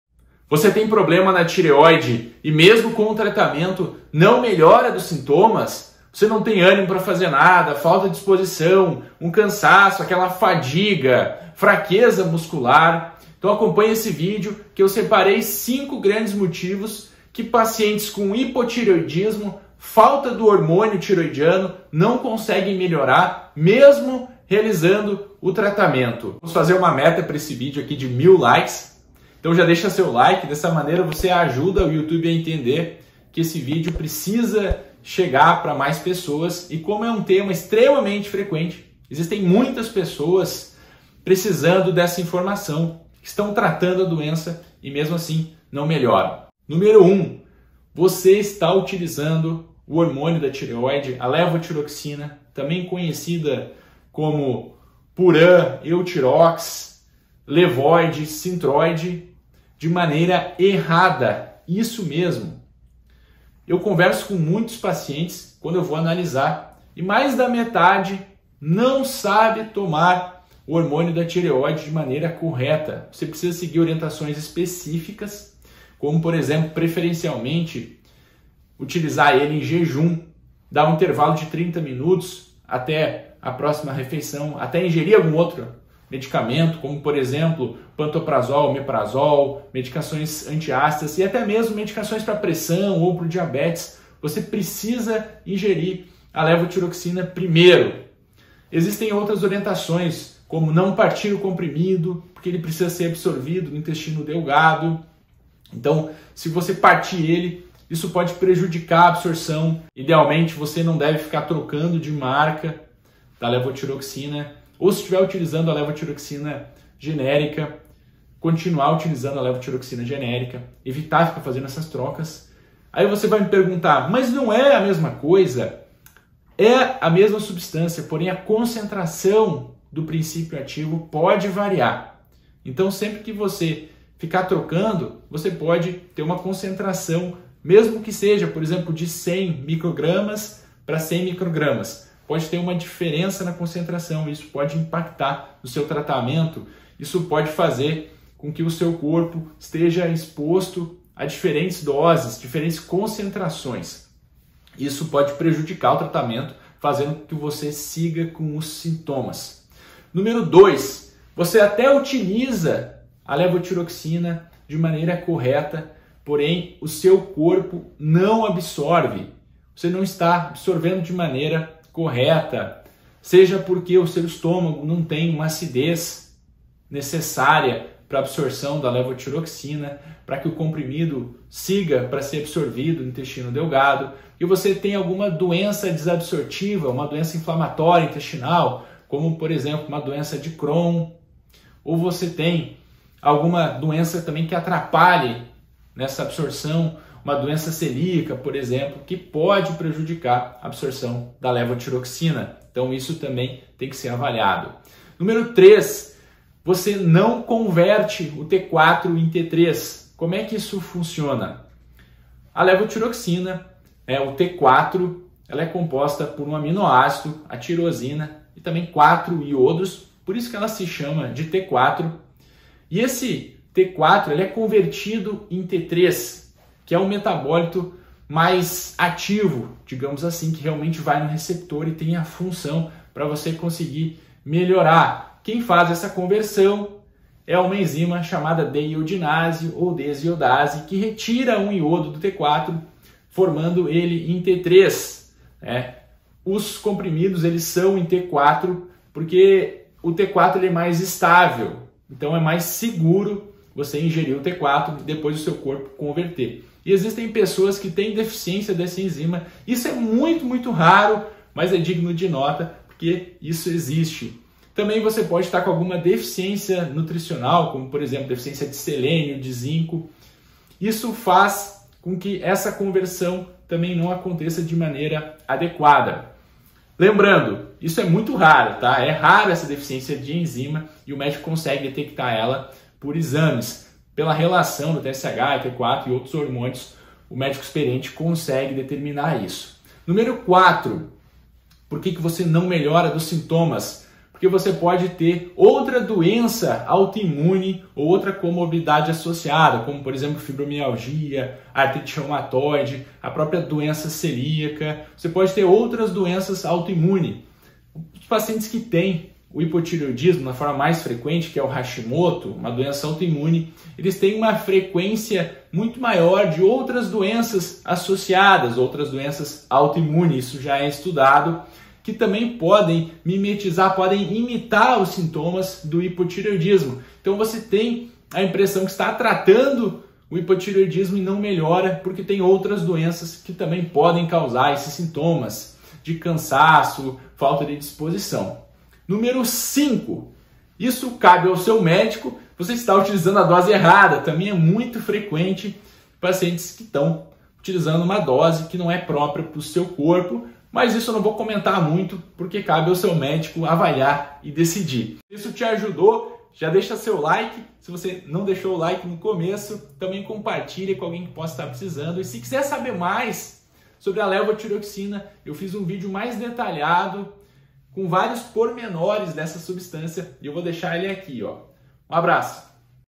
Sintomas de hipotireoidismo persistentes?
Você tem hipotireoidismo e não melhora dos sintomas mesmo com tratamento?
Separei 5 principais causas que podem fazer você não melhorar dos sintomas do hipotireoidismo mesmo que faça o tratamento com Levotiroxina genérico ou com Puran T4, Levoid, Synthroid ou Euthyrox.
Com o tratamento do hipotireoidismo você deve melhorar completamento dos sintomas, eles não devem ser persistentes.

Índice do vídeo:
0:00 Inscreva-se
0:10 Introdução
0:20 Sintomas de hipotireoidismo persistentes?